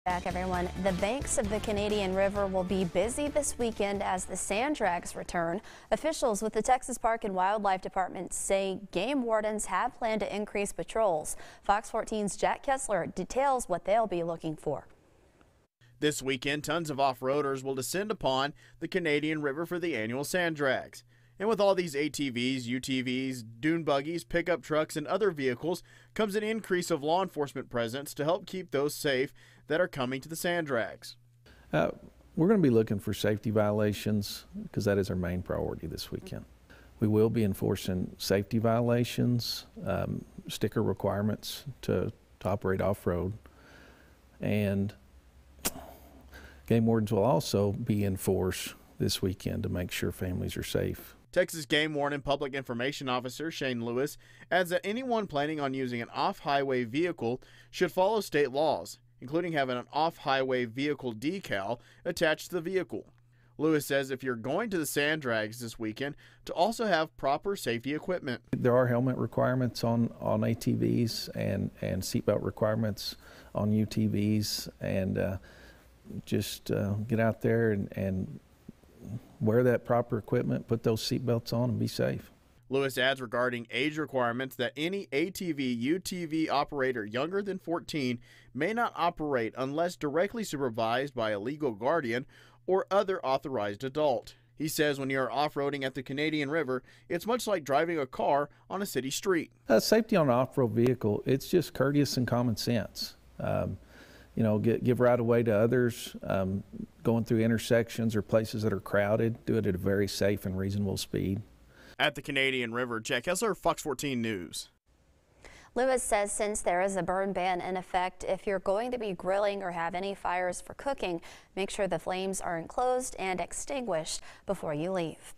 Game Wardens increase patrol ahead of Sand Drags event
According to officials with the Texas Parks & Wildlife Department (TPWD), Game Wardens have planned to increase patrols for the upcoming Sand Drags event along the Canadian River.